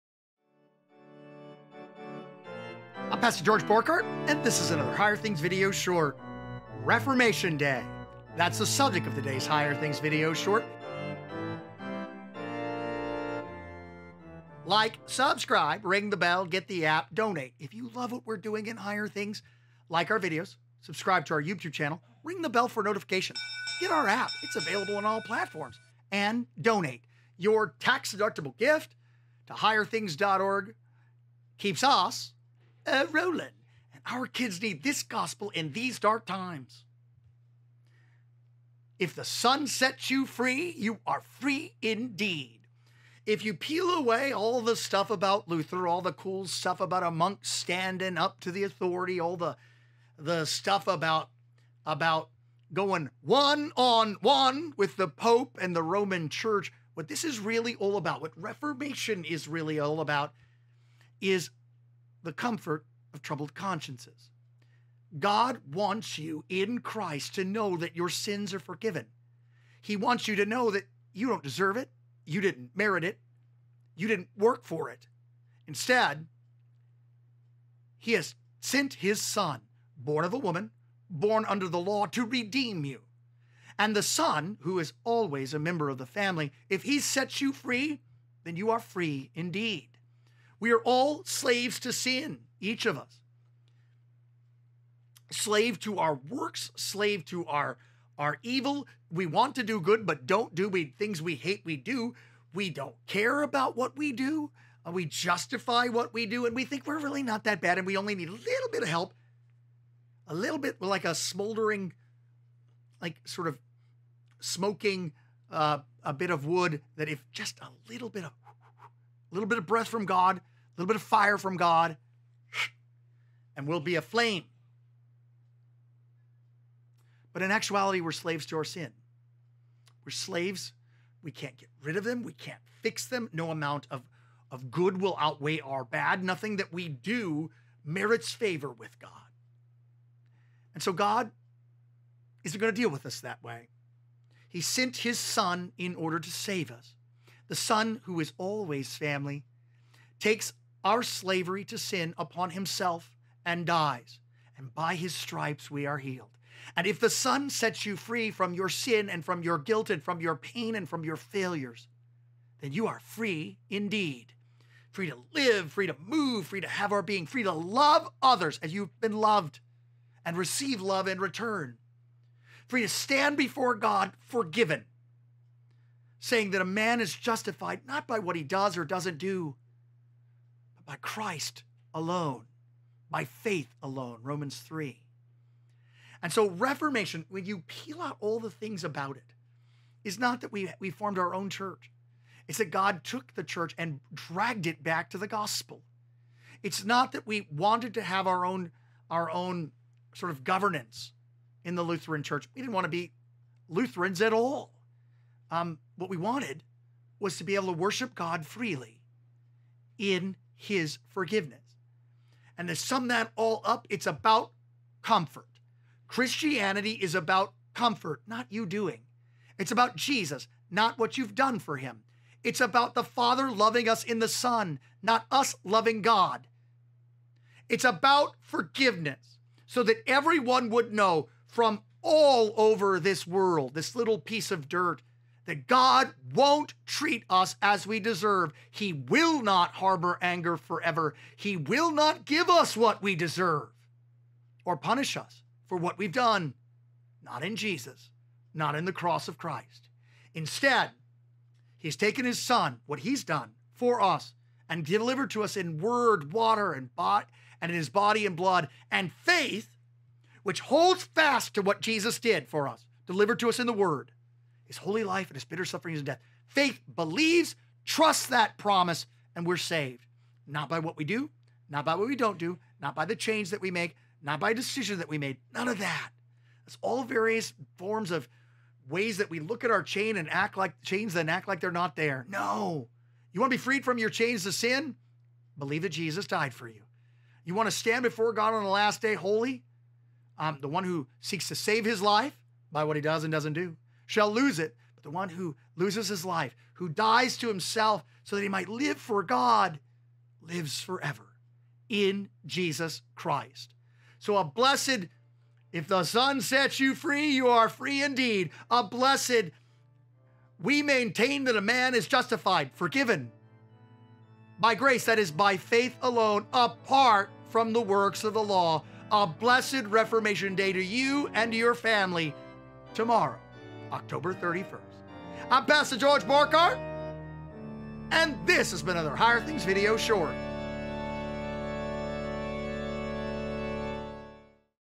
The Why of the Reformation in 5 Minutes - A Higher Things® Video Short
What was the Reformation really about? Here’s your five minute rundown!